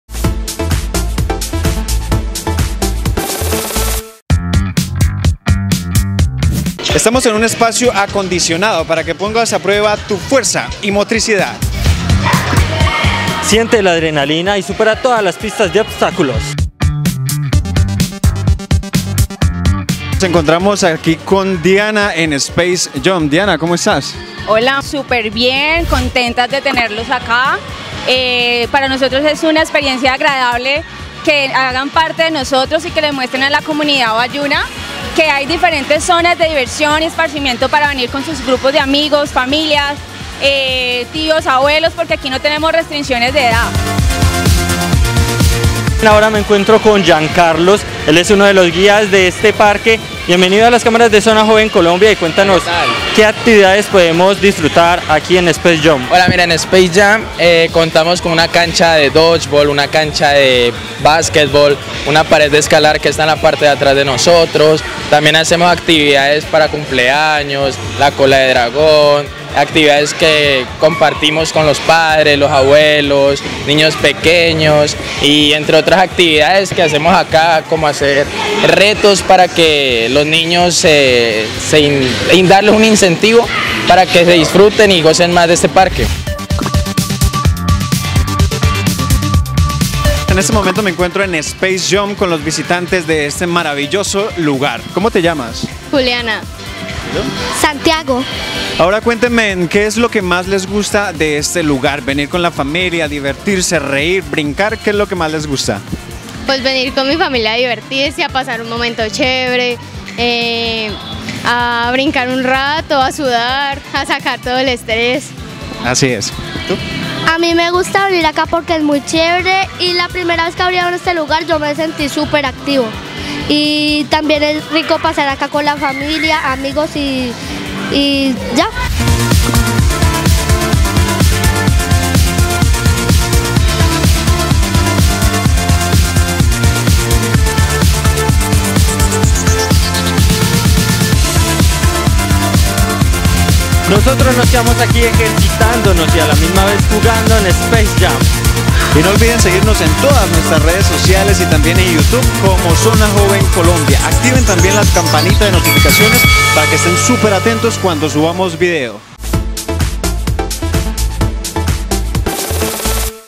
Space Jump Cali, diversión asegurada
Cali tiene un lugar donde los grandes juegan como niños y los niños como grandes. Se trata de una iniciativa local que ofrece, a través de un parque de trampolines, diversión desde los 3 a los 100 años. Cónoce este divertido lugar con Zona Joven Colombia